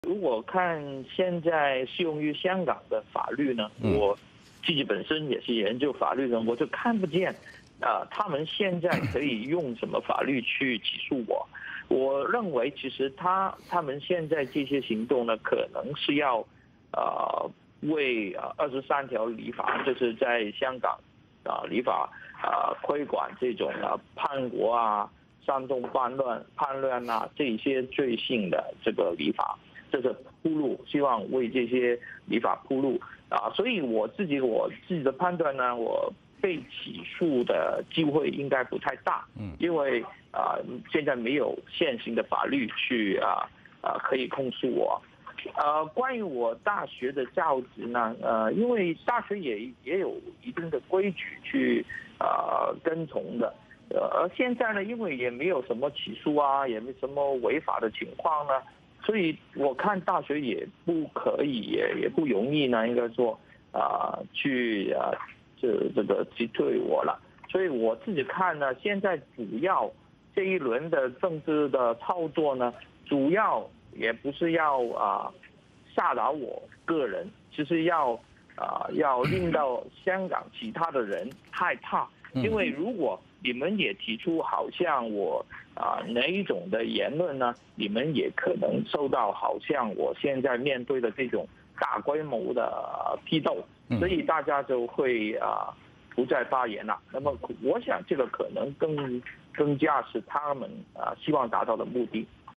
【戴耀廷：立我典型 杀鸡儆猴】4/4
戴耀廷：我看现在适用于香港的法律，我自己也是研究法律的，我看不到他们可以用什么法律来起诉我。他们现在的行动可能是为23条立法、叛国啊等罪行的立法铺路。我判断我被起诉的可能不太大，因为没有现行的法律可以控诉我。关于大学的教职，大学也是有规矩的。现在没有起诉和违法的情况，大学也不可以容易地辞退我。我看这一轮的政治操作，主要也不是为了吓到我个人，而是为了让香港其他人害怕。因为如果你提出和我类似的言论，你可能还会受到大规模批斗。我想这是他们想要达到的目的。
【VOA连线(戴耀廷)：遭北京和港府猛批，戴耀廷有话要说】